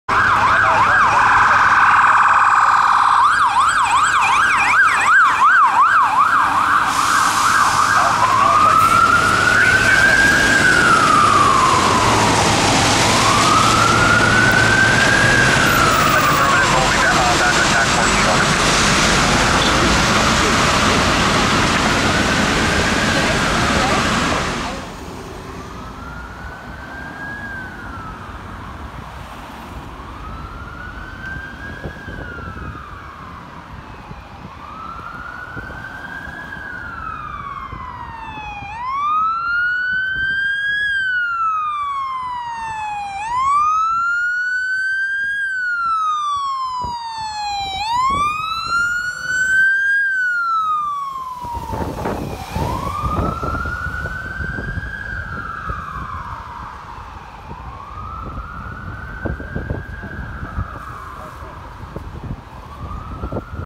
*NEW SIREN* BC Ambulance Service - Unit 63216 Responding (x2)
2 Clips I combined into 1 of BC Ambulance Service Emergency Paramedic (BLS) Unit 63216, Responding to separate calls. This is 1 out of the 2 units out of the whole BC Ambulance Service, fleet that have the new siren which I believe is a Carson.

Types of calls:
1st Clip - Transporting a Patient to St.Pauls Hospital
2nd Clip - Medical Call with Pump 6

BCAS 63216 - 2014 Chevy Demers

Videos Taken - Dec 18th & Dec 19th 2017